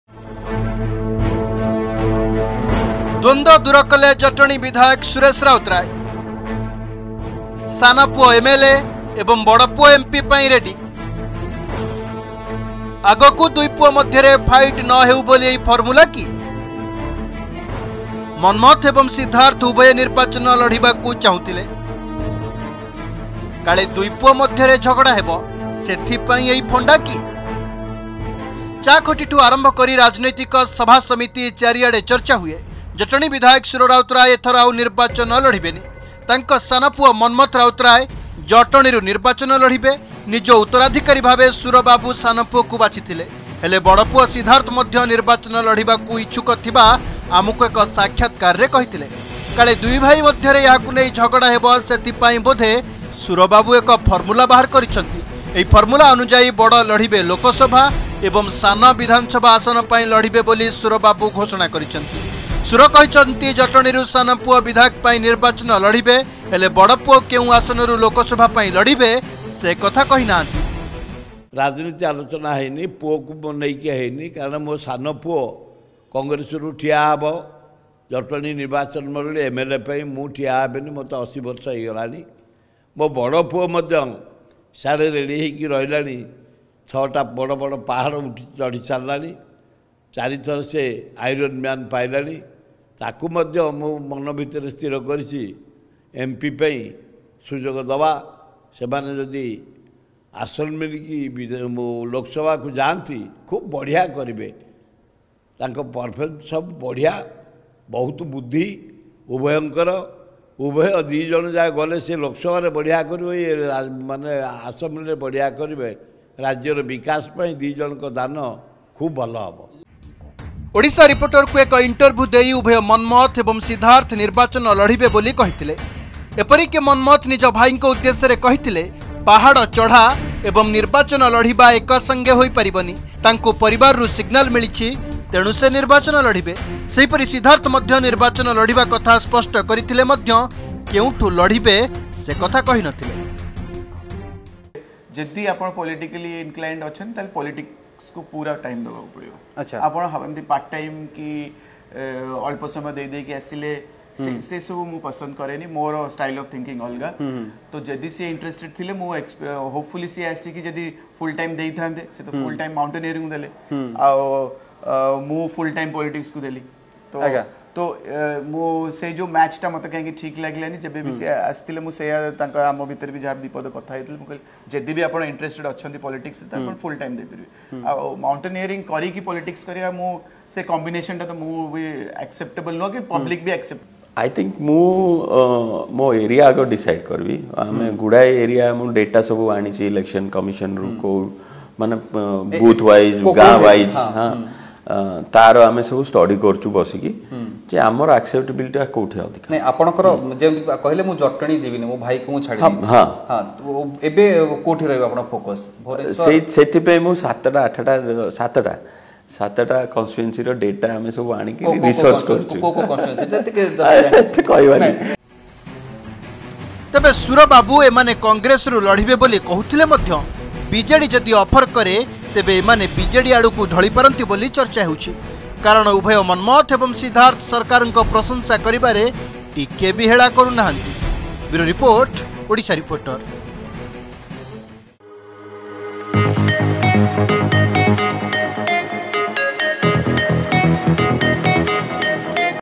କଳିର ସୁତ୍ର କାଢ଼ିଲେ ସୁର | Sura Routray Find Out A formula To End Conflict betwn 2 Son। Odisha Reporter
ସାନ ପୁଅ ଏମଏଲଏ ଏବଂ ବଡ଼ ପୁଅ ଏମପି ପାଇଁ ରେଡି। ଆଗକୁ 2 ପୁଅ ମଧ୍ୟରେ ଫାଇଟ୍ ନ ହେଉ ବୋଲି ଏହି ଫର୍ମୁଲା କି ? ମନ୍ମଥ ଏବଂ ସିଦ୍ଧାର୍ଥ ଉଭୟ ନିର୍ବାଚନ ଲଢିବାକୁ ଚାହୁଁଥିଲେ । କାଳେ ଦୁଇ ପୁଅ ମଧ୍ୟରେ ଝଗଡ଼ା ହେବ ସେଥିପାଇଁ ଏହି ଫଣ୍ଡା କି ?